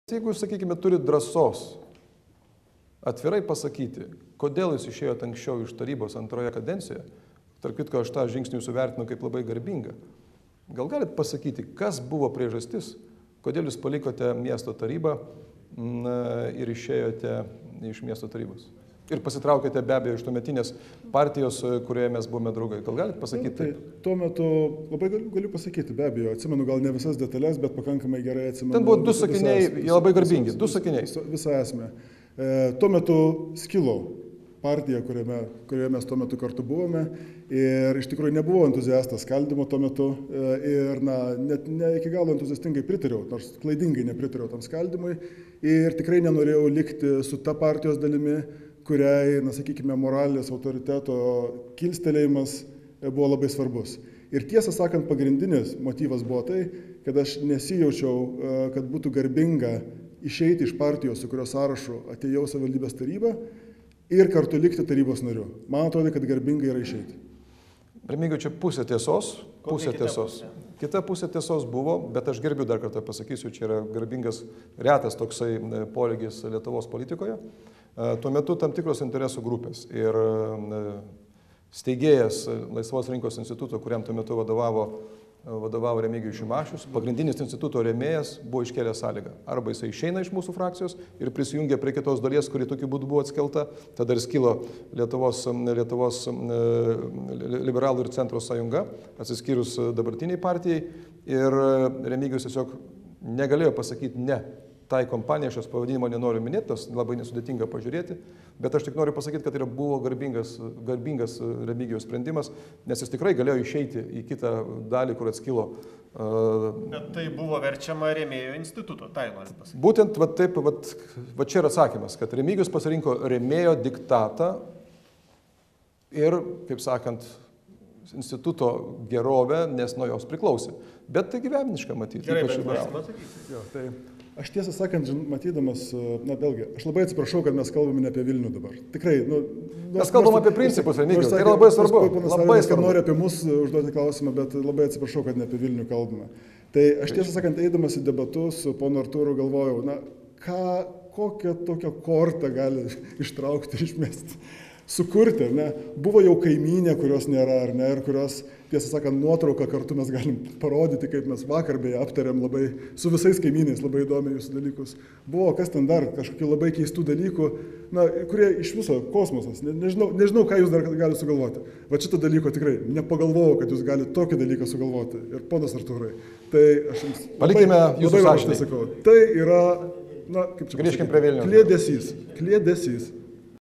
Kodėl R. Šimašius buvo palikęs Vilniaus tarybą?
Artūro Zuoko versija, kodėl Remigijus Šimašius 2006 metais paliko Vilniaus tarybą.